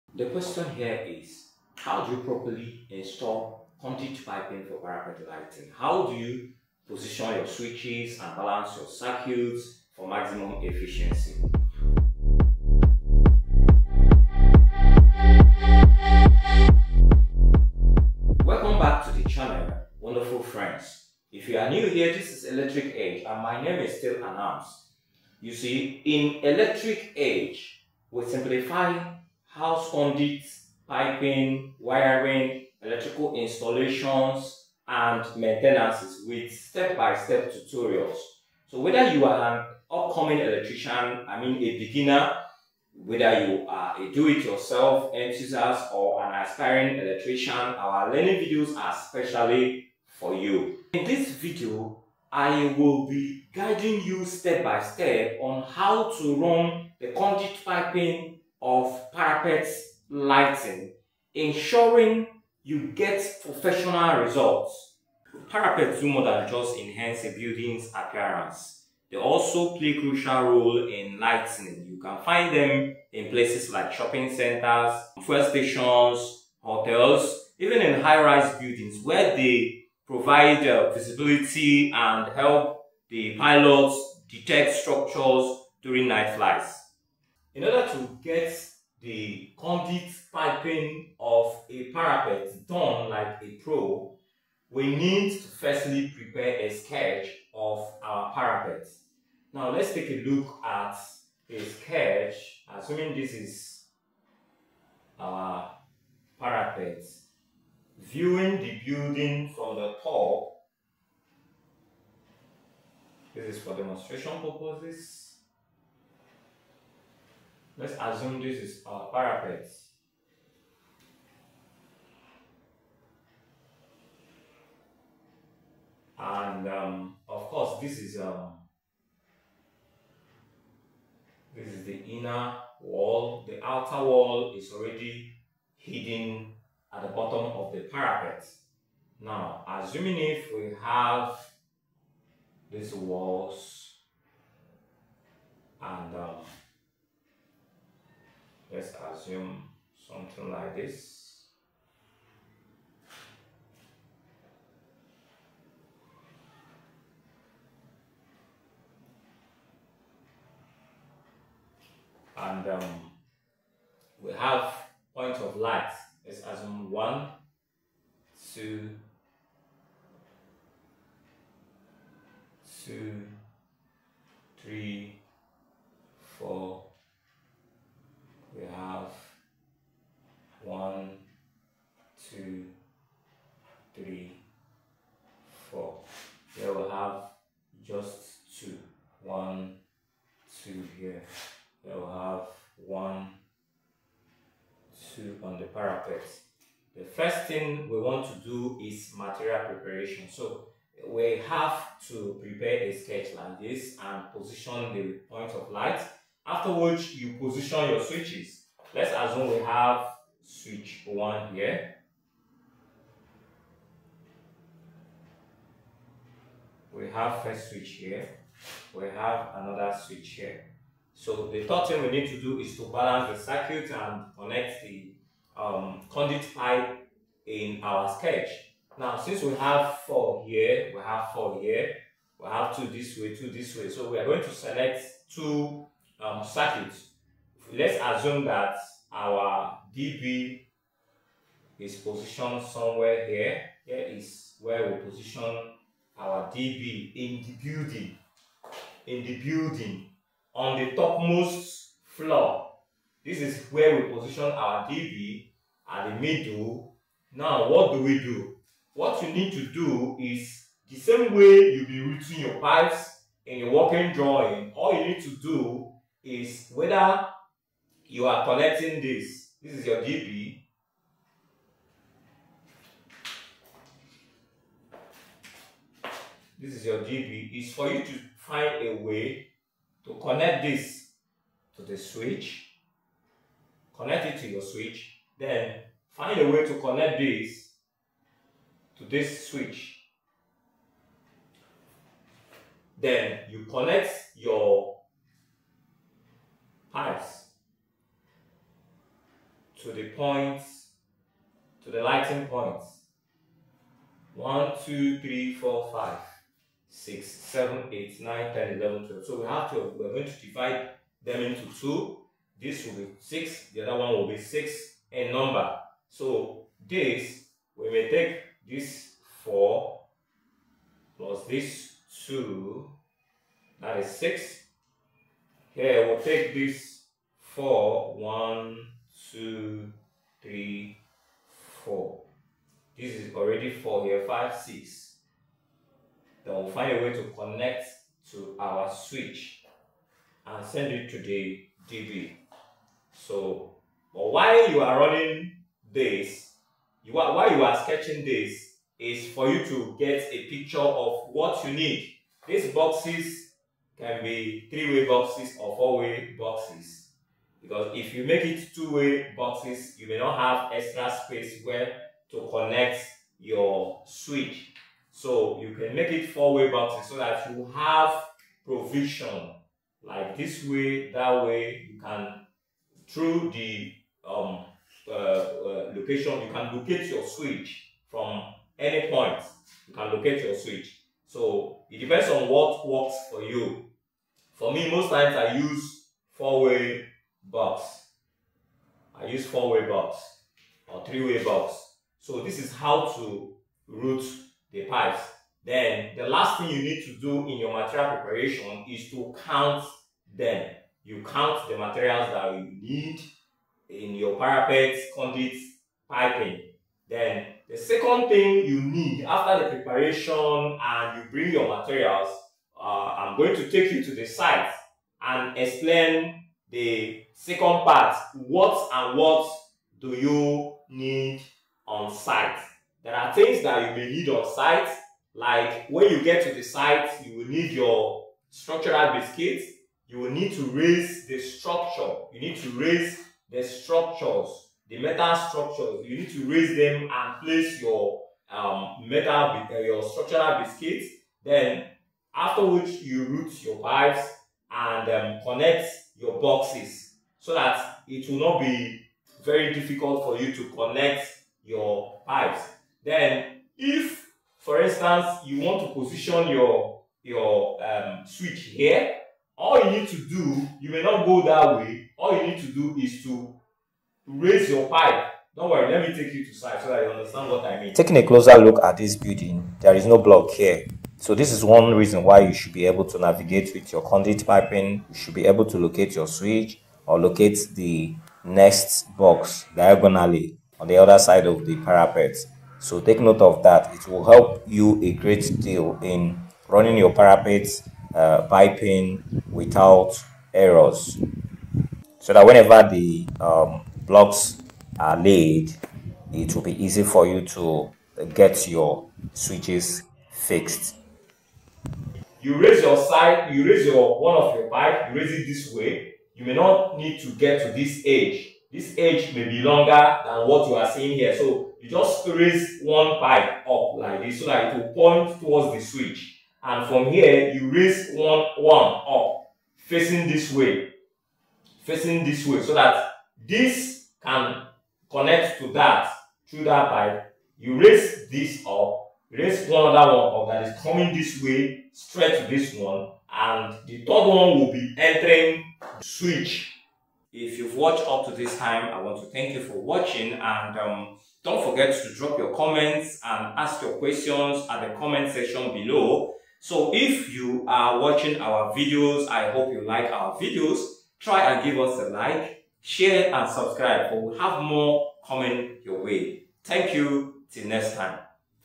How to do Conduit Piping of Parapets | Step-by-Step Guide for Proper Installation
Parapets do more than just enhance a building’s aesthetics—they also play a crucial role in outdoor lighting design. You’ll often see them in fuel stations, shopping centers, hotels, and high-rise buildings, where they improve visibility and help pilots detect structures during night flights.

In this video, I’ll walk you through the conduit piping process for parapet lighting, including:
✅ How to install conduit pipes correctly
✅ Proper switch positioning for easy control
✅ Balancing lighting circuits for efficiency

Whether you're an electrician, contractor, or DIY enthusiast, this guide will help you achieve professional results. Don't miss out!